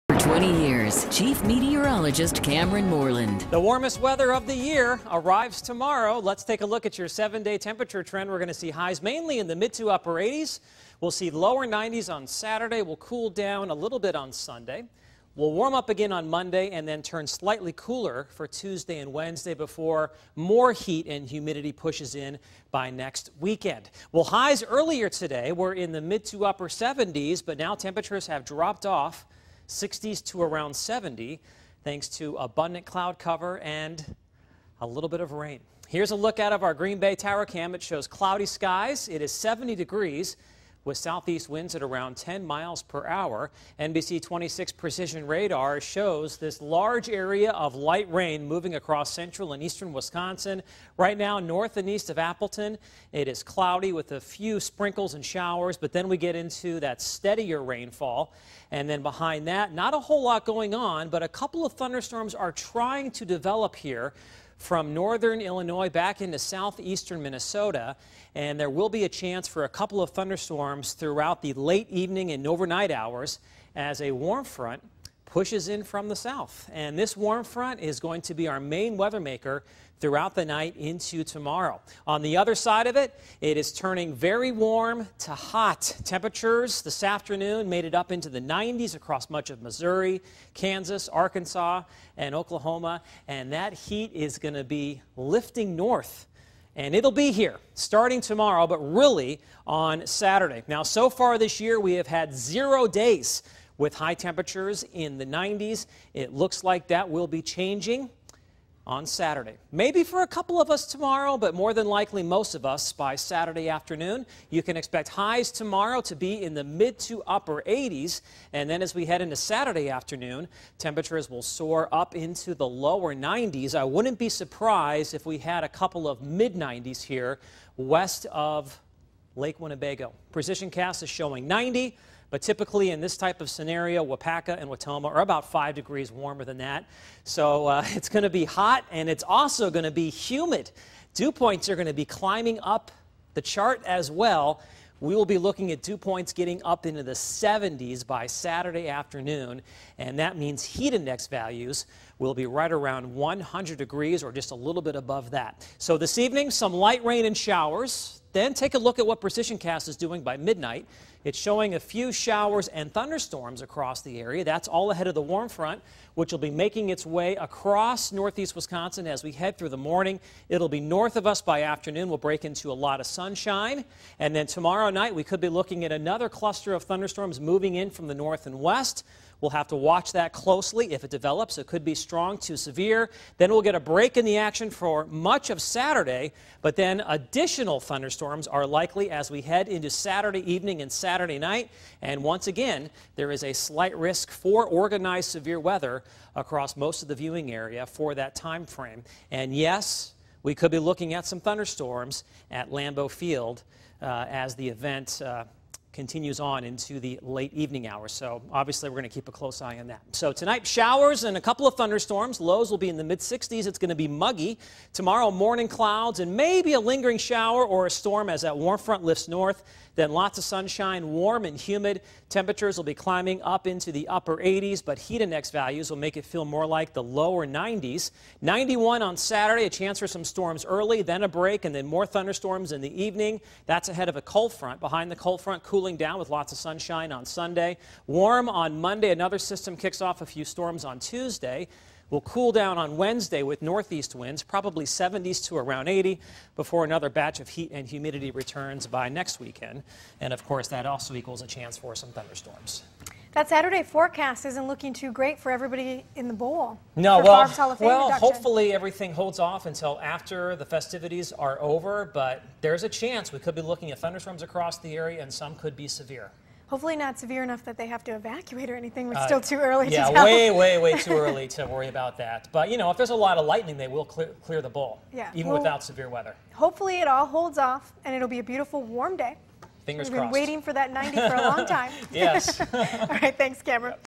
Cameron's Forecast
Heat and humidity are back in the forecast.